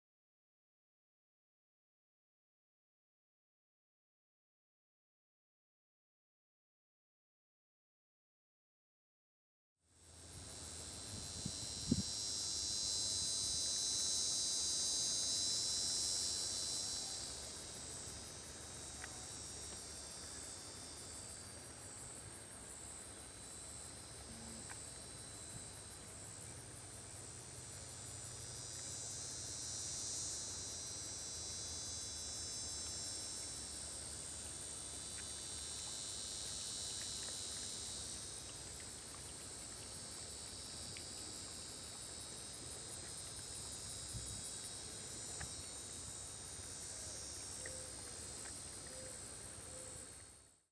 Grey Fox - Release
A juvenile female ‪#‎GreyFox‬ (#16-1895) was brought to ‪#‎CROWClinic‬ after it was caught in a raccoon trap in Lehigh Acres. The fox arrived stressed, dehydrated and has remained quiet, alert and responsive. She ate well and enjoyed a diet of mice and fish and was  released on June 30, 2016.